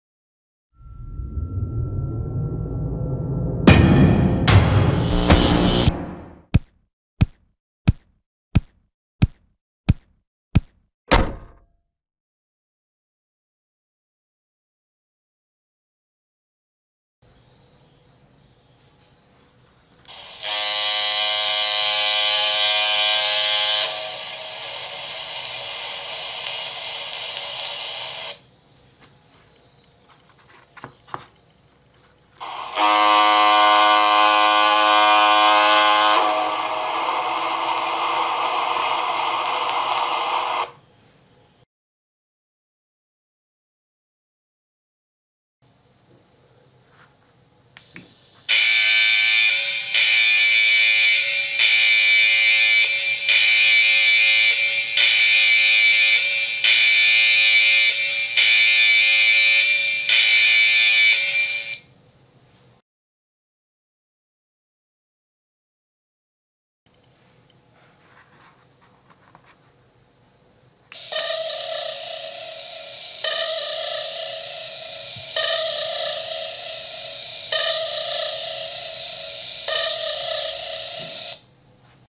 Exemples des modules son de bateaux RC de la marque Krick
Exemples des modules son de bateaux RC de la marque Krick: un module "corne de brume, un module "Alarme" et un module "Ping".
Ces modules sont disponibles sur le site Tecnimodel.com